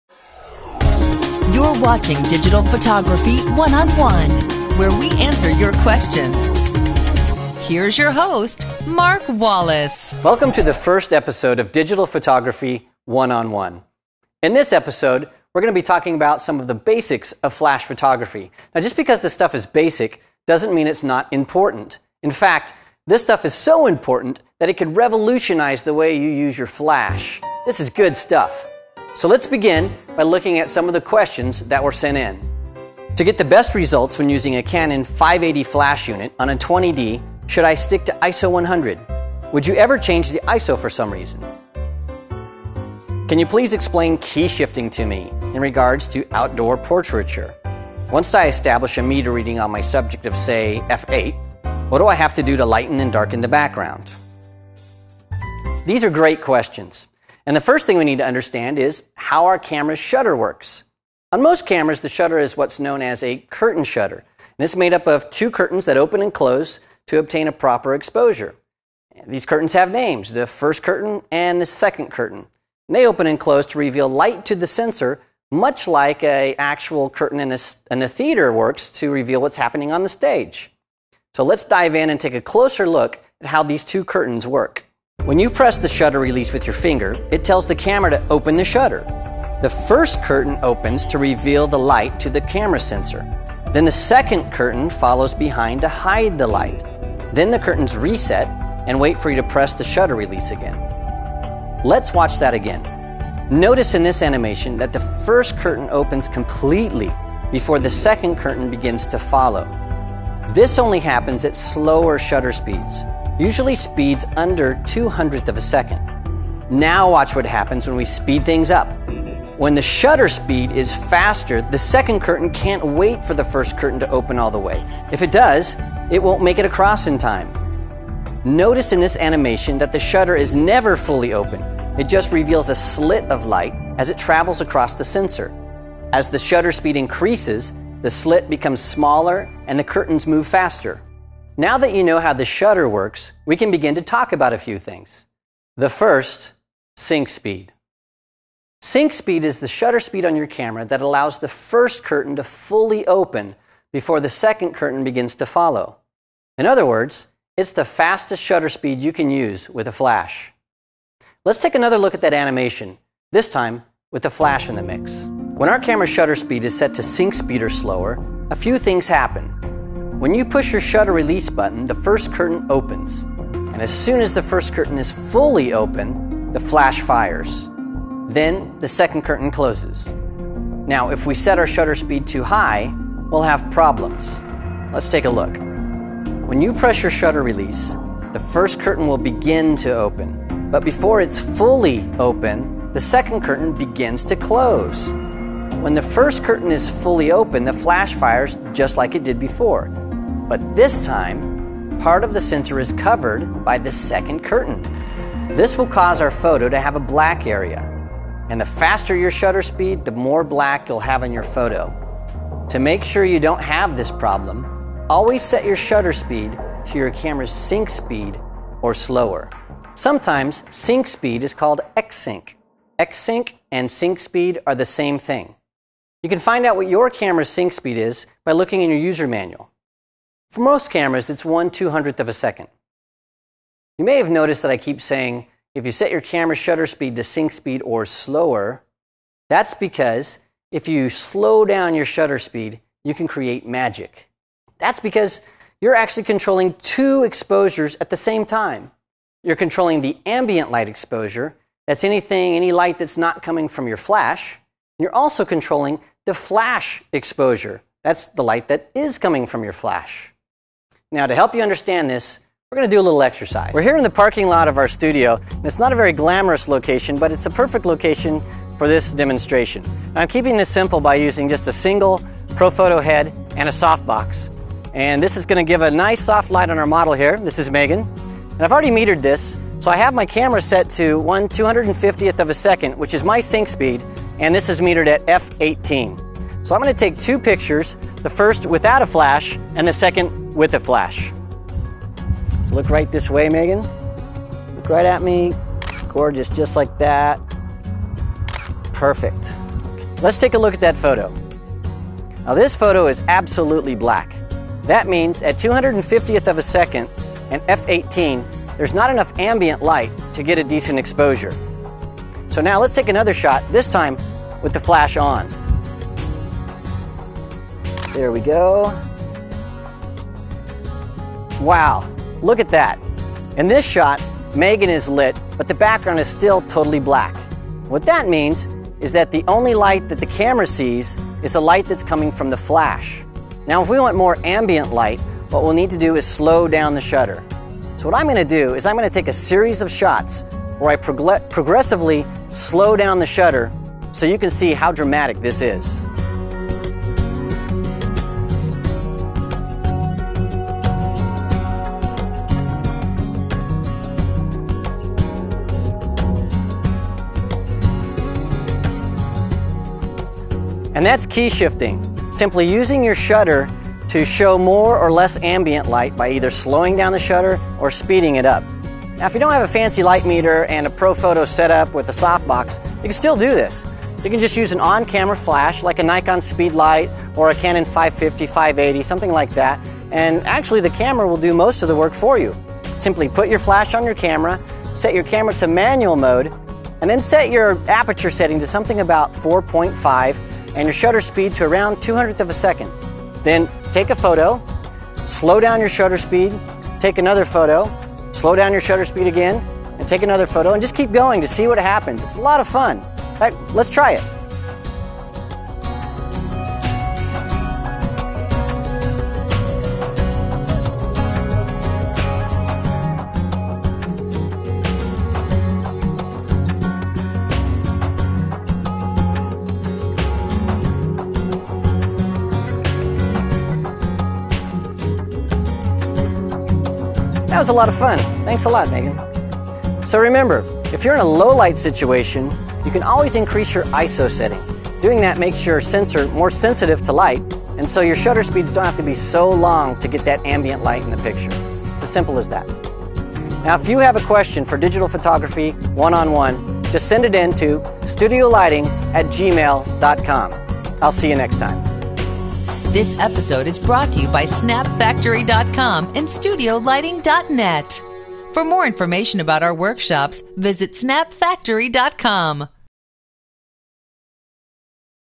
Digital Photography 1 on 1: Episode 1
In this episode Mark explains how the shutter works, shutter sync, and how to control the ambient light exposure while using a flash.